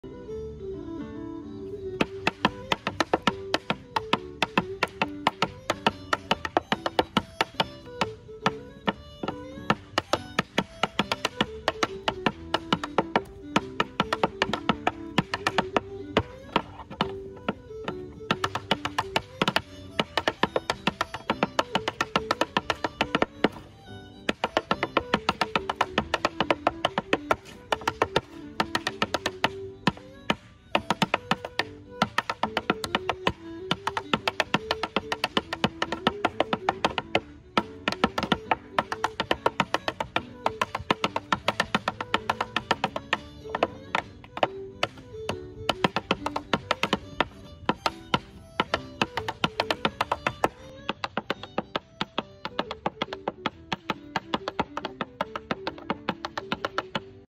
CLRG Cornphíopa le Chéile challenge
Here’s something that’s a little bit different from my usual dancing videos ✨
I had so much fun coming up with 32 bars of traditional hornpipe steps for the CLRG Cornphíopa le Chéile challenge! I encourage all Irish Dancers to take part in this competition because not only is it fun to come up with your own original choreography, but it’s just so important that we embrace the traditional side of Irish Dance as well as the newer, modern steps that we typically do on the competition stage.